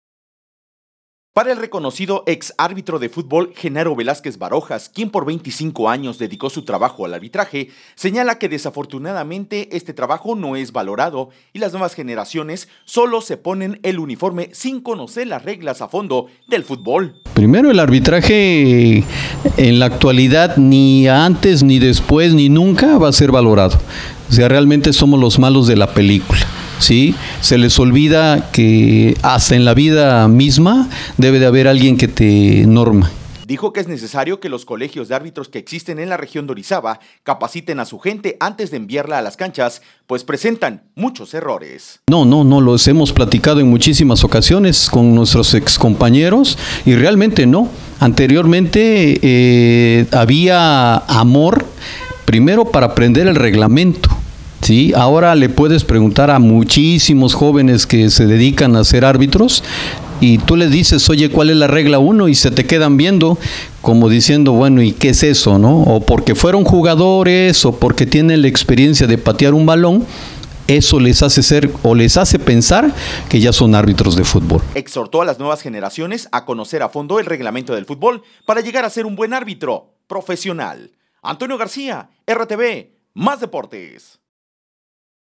Necesario que se reconozca el trabajo del árbitro de fútbol
Genaro Velázquez Barojas quien por 25 años dedico su vida al arbitraje, señala que desafortunadamente ese trabajo no es valorado.